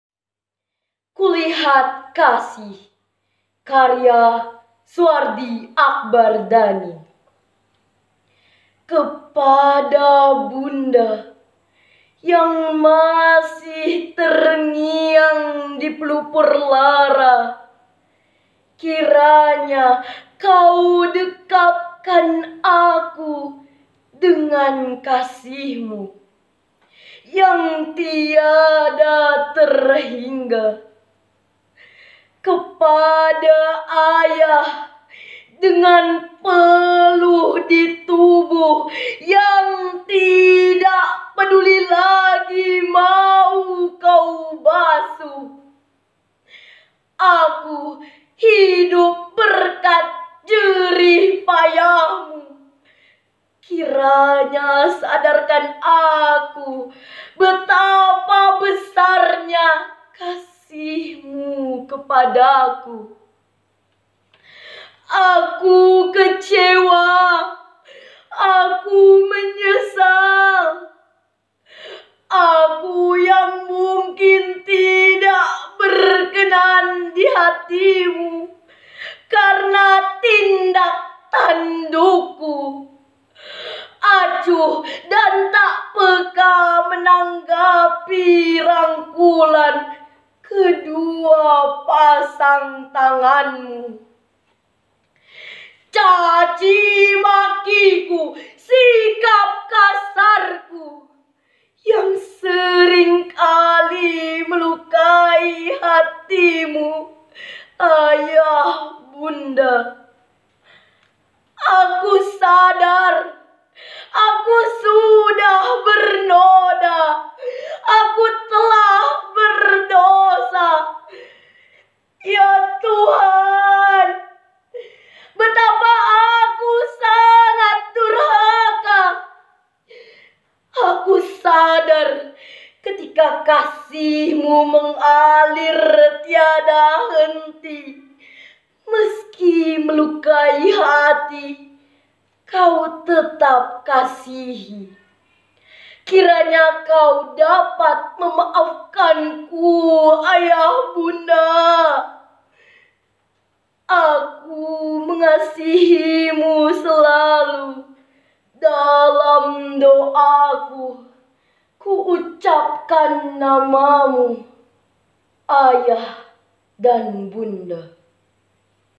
Nazwa Salma
Lomba Membaca Puisi Tingkat SMP
Asal sekolah : SMP N 1 Sekadau Hilir